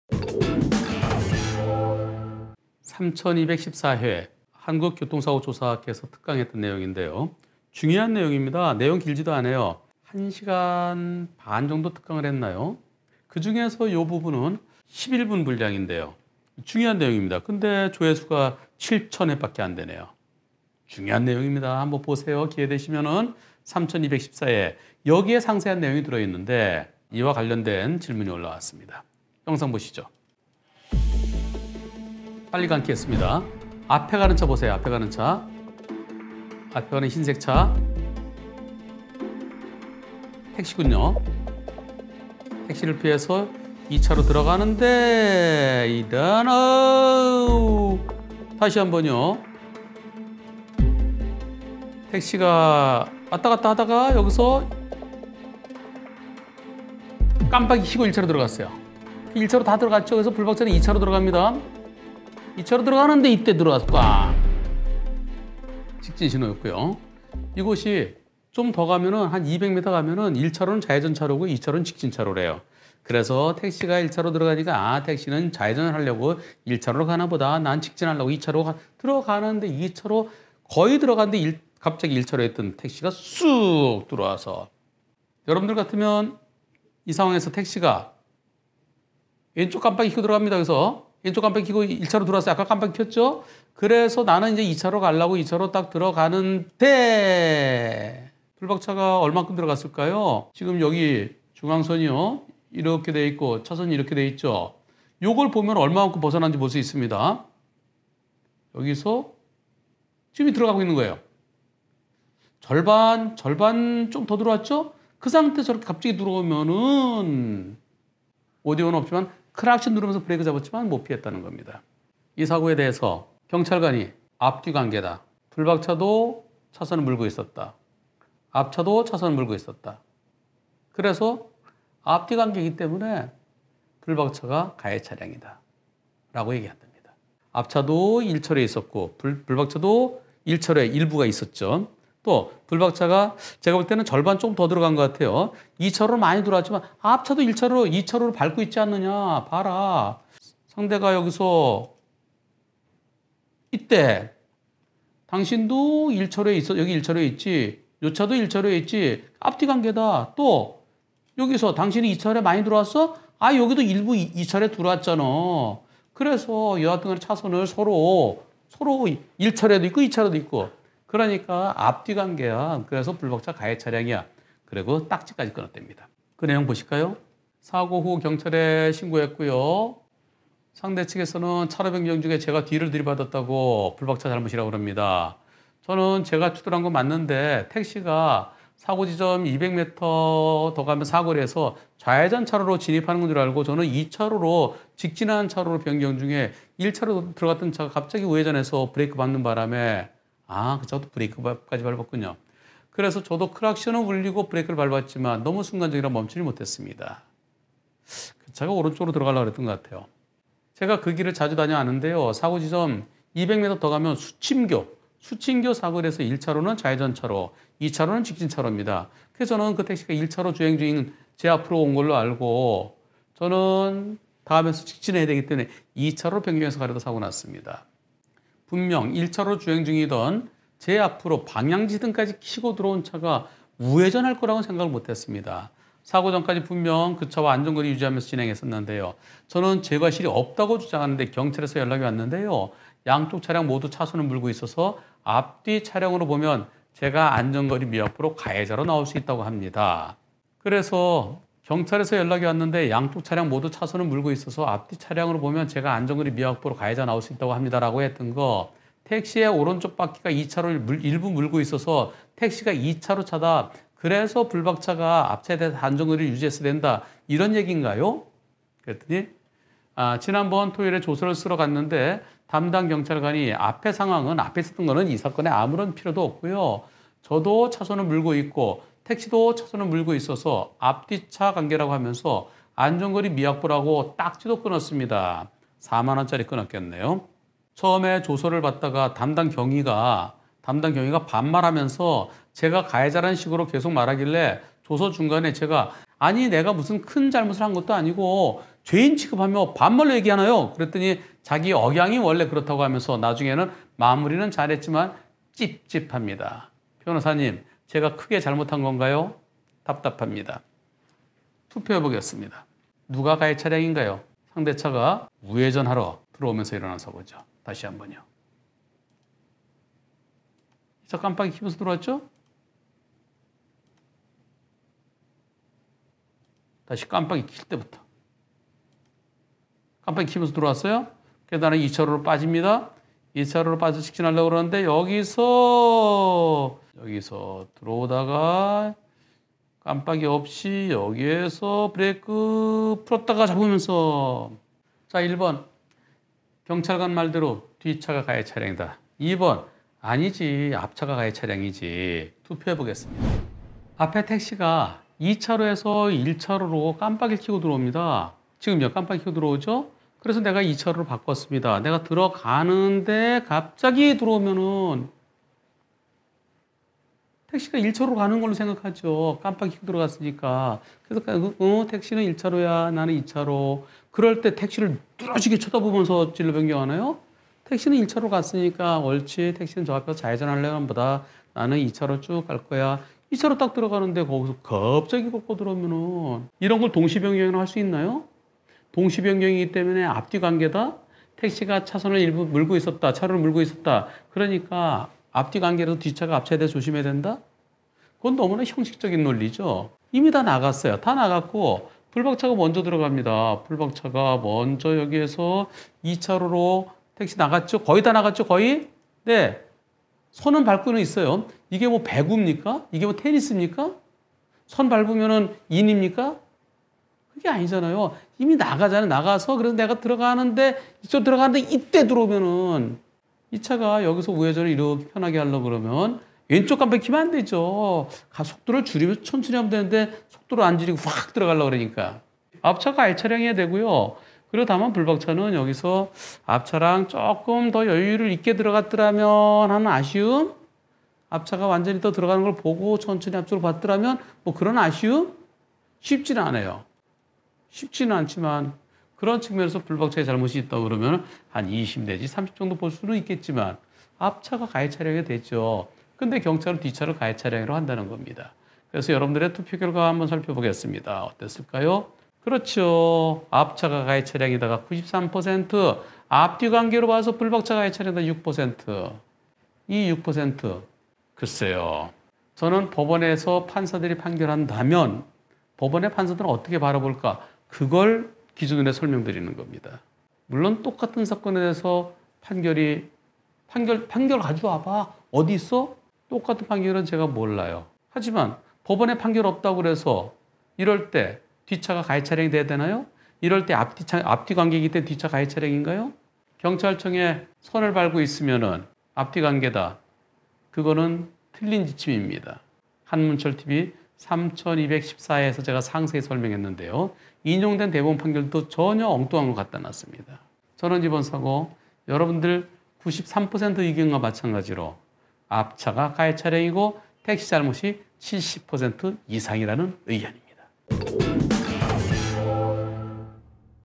4257회. (4207회 투표결과) 깜빡이 켜고 1차로로 들어간 택시를 피해 2차로로 차로변경하는데 1차로로 90% 이상 들어갔던 택시가 갑자기 다시 우회전?
(b5658, u4207, u3214, 택시 잘못 70% 이상 의견, 투표결과 93:6으로 택시가 가해차량이라는 의견이 절대 다수, 가히, 블랙박스, 블박영상, 교통사고 영상, traffic accident, car accident, dash cam, black box)